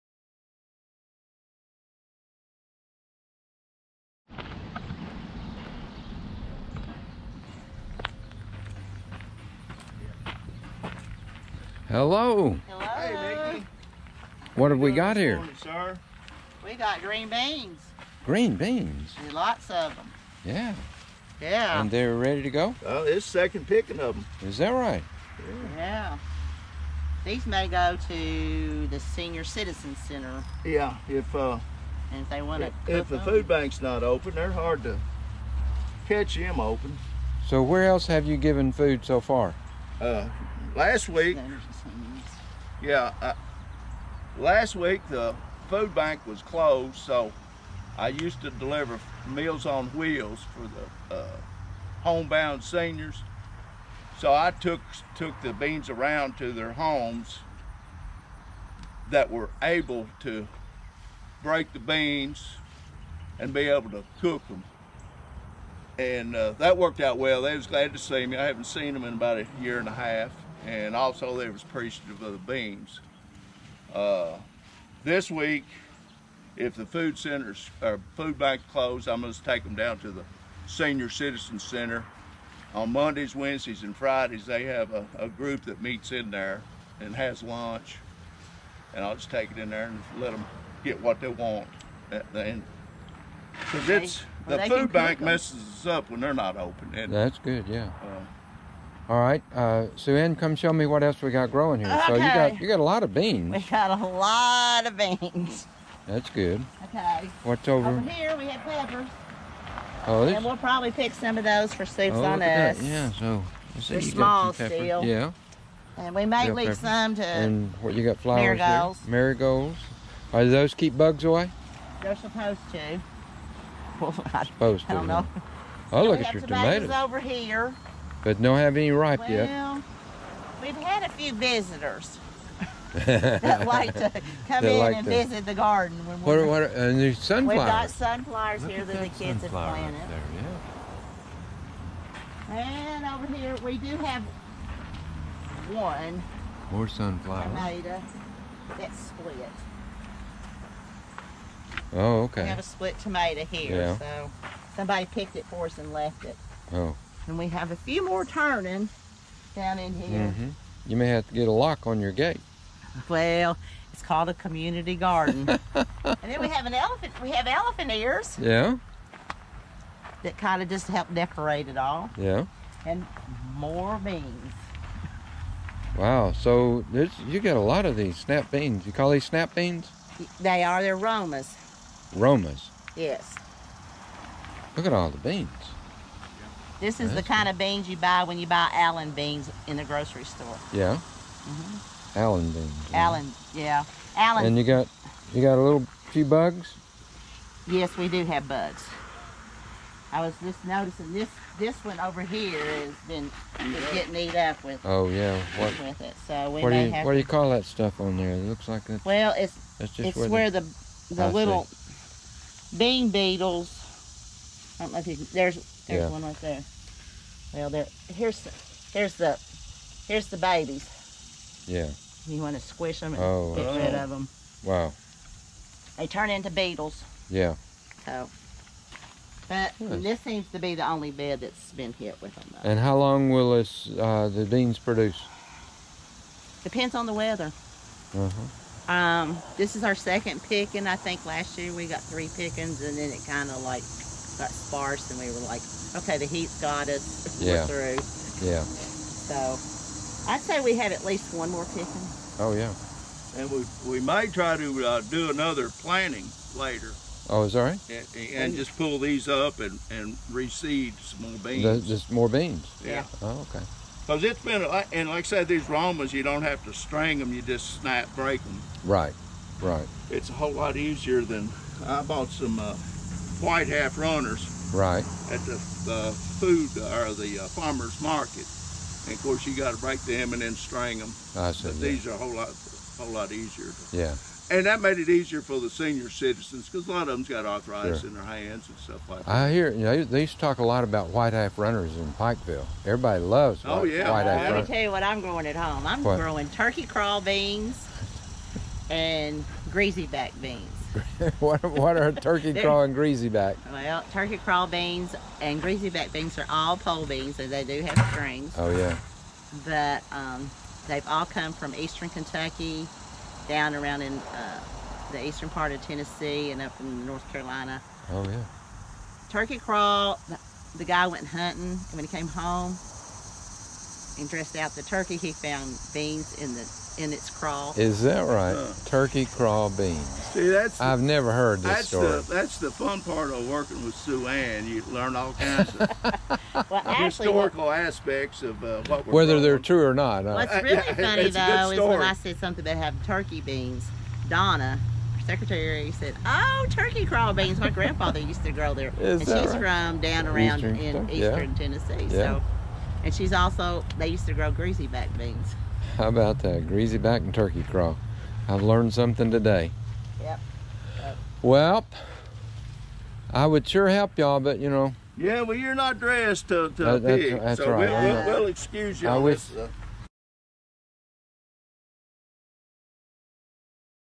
Chats With Mickey
In this episode Mickey visits SueAnn and Steve while they are harvesting beans in Grace Gardens.